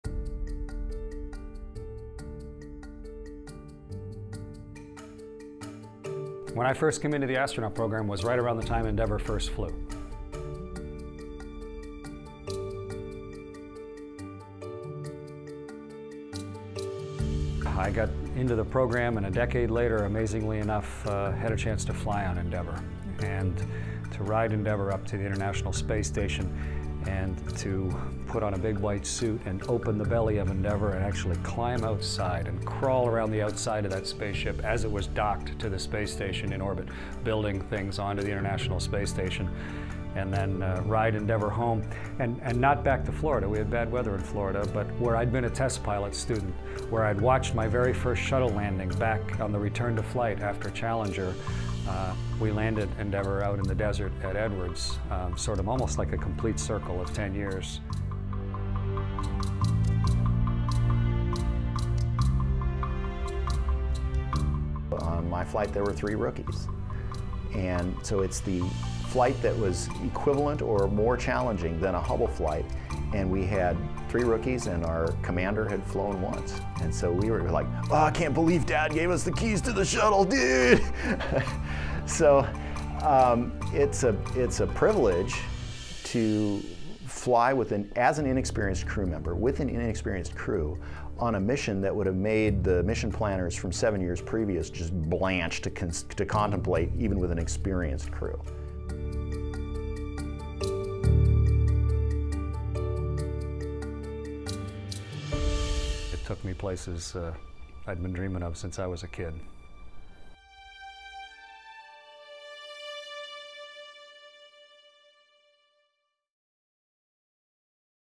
Living Space: Astronauts Chris Hadfield and Stan Love
Astronauts Chris Hadfield and Stan Love discuss their flights on space shuttle Endeavour.